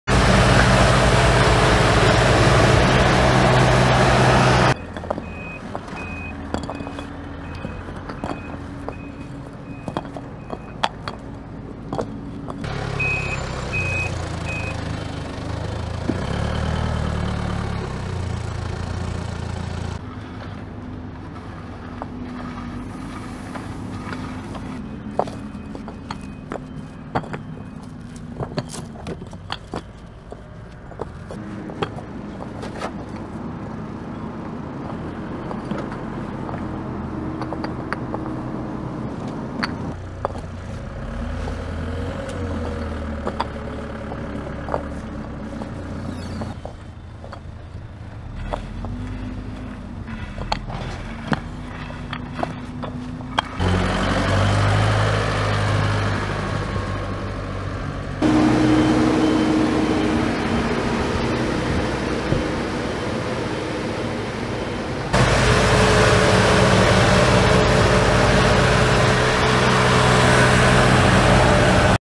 Doğukent'te yeni yollar kilitli parkeyle döşeniyor
Karaköprü Belediyesi ilçe genelinde üstyapı çalışmalarını aralıksız sürdürürken, Doğukent Mahallesinde yeni açılan yollarda kilitli parke döşeme çalışması gerçekleştirerek hizmete sunuyor.